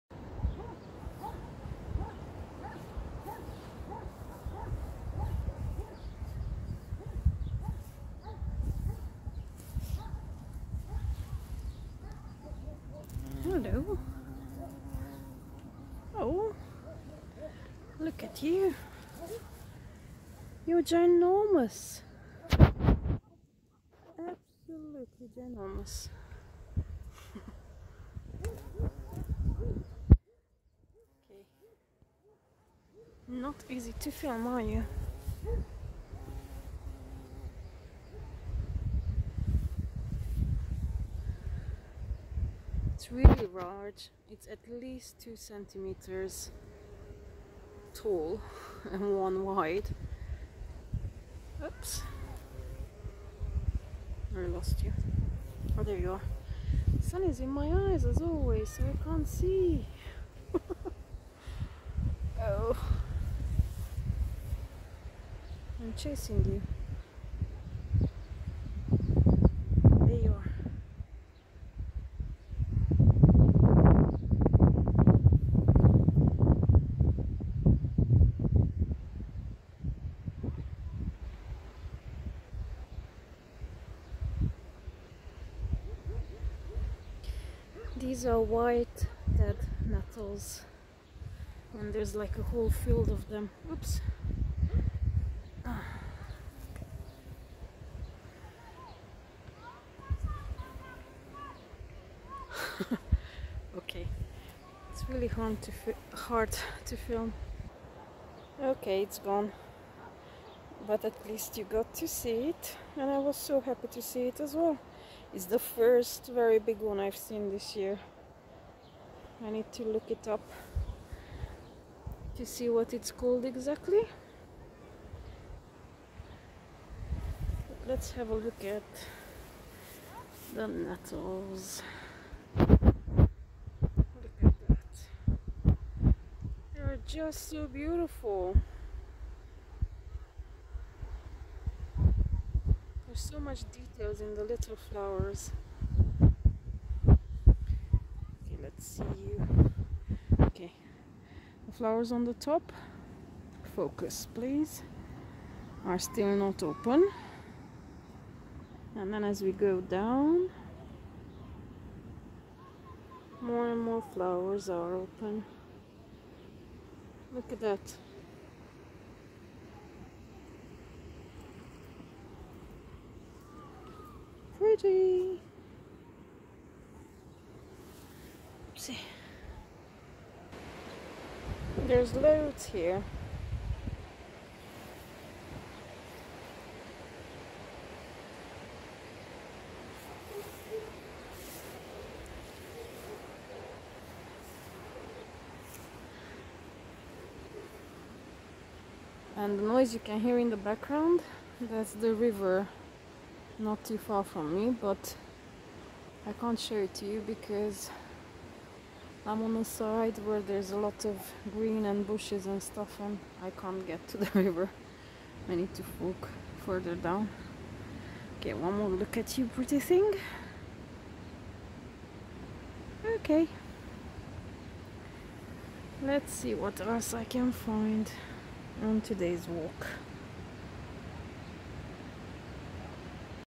Pretty white dead-nettles and a very large bumble bee 🐝
This is another example of harmony in nature: flowers full of pollen and nectar and their pollinator, a cute bumble bee. They work together and they sustain each other. A lesson we can learn from them, right?